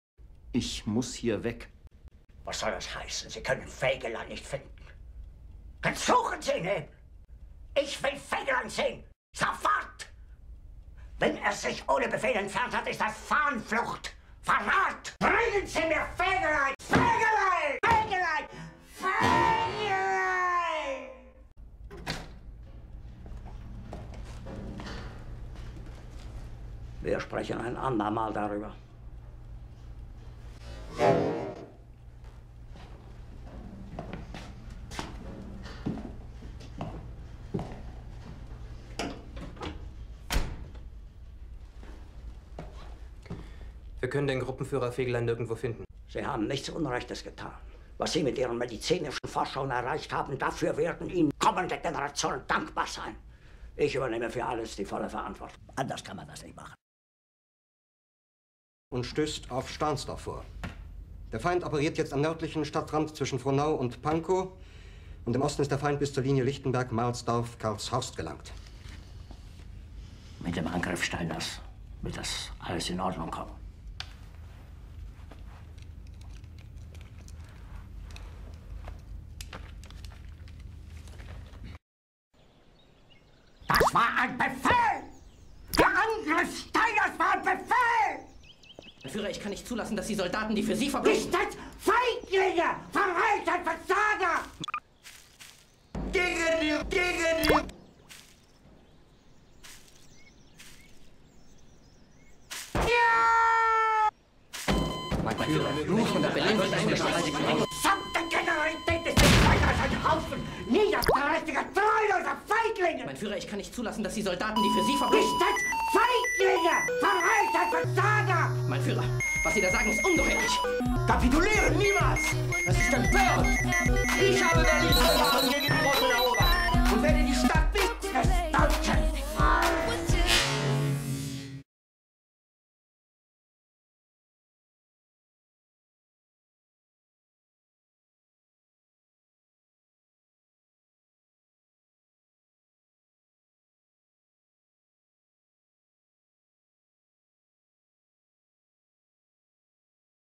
Hitler plays the FegelTari 2013
Hitler has a discussion with Grawitz and Guncshe about playing the FegelTari 2013
When hitler plays it unexpected things happen.Discover them in the video